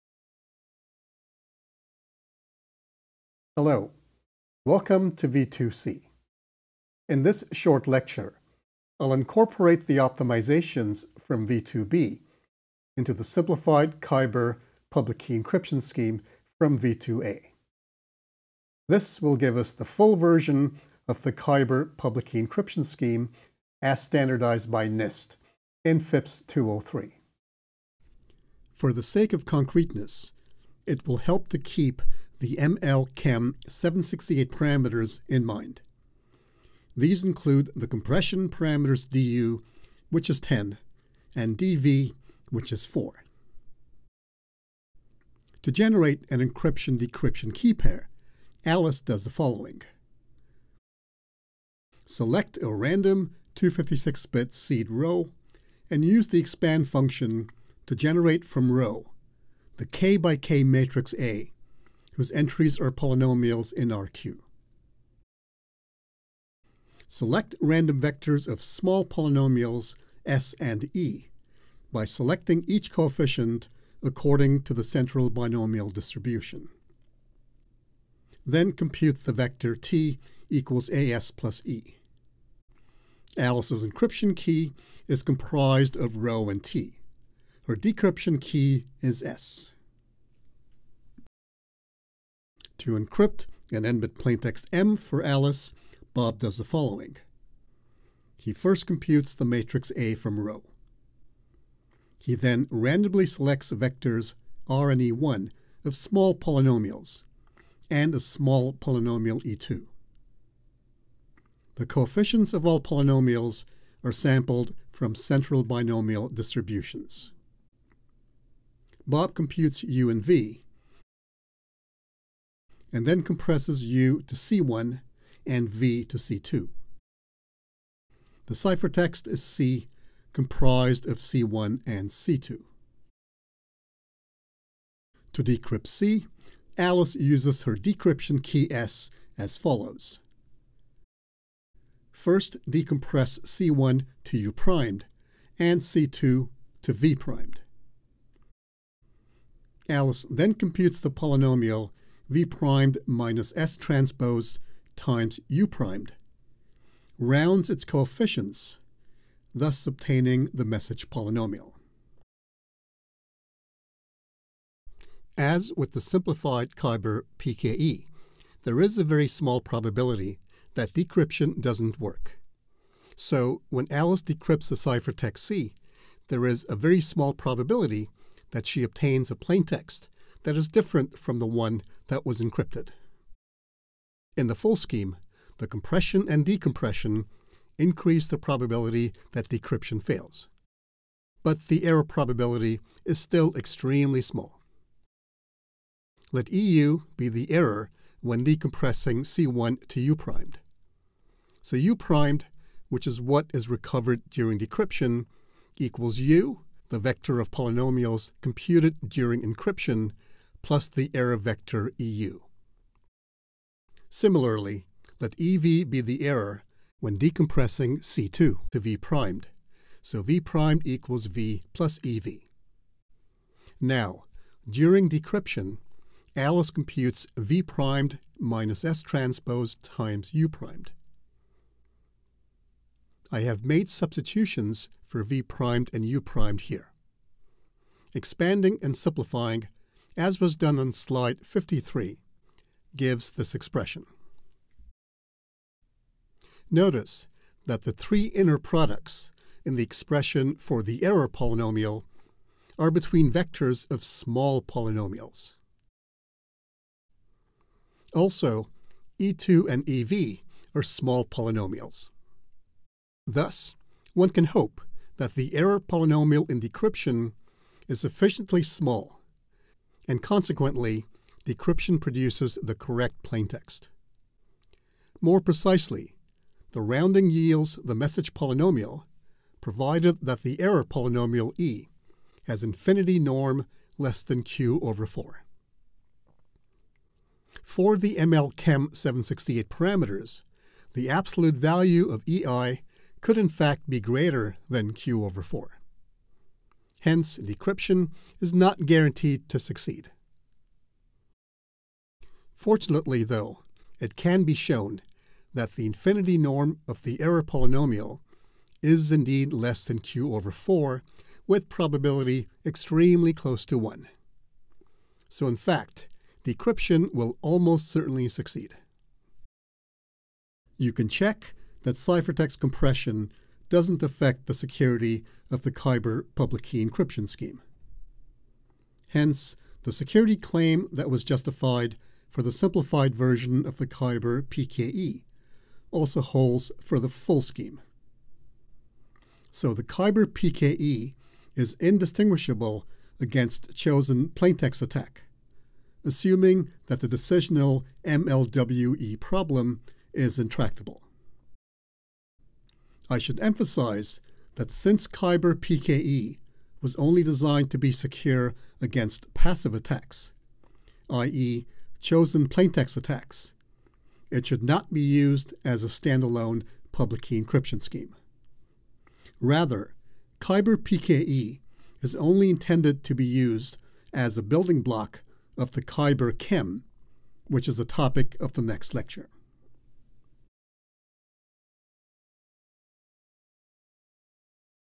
V2c: Kyber-PKE (full scheme) (Kyber and Dilithium short course)
Video lectures for Alfred Menezes's introductory course on Kyber-KEM (ML-KEM) and the Dilithium signature scheme (ML-DSA). These lattice-based cryptographic schemes were standardized by NIST on August 13, 2024.

Topics covered: Kyber public-key encryption scheme, lattice-based cryptography

The slides are available on the course web page.

Slides
00:00 Introduction
00:27 Slide 65: Domain parameters and key generation
01:30 Slide 66: Encryption and decryption
02:42 Slide 67: Decryption doesn't always work
05:18 Slide 68: Security

Corrections:
01:10 e is selected from S_{\eta_1}^k (not from S_{\eta_2}^k)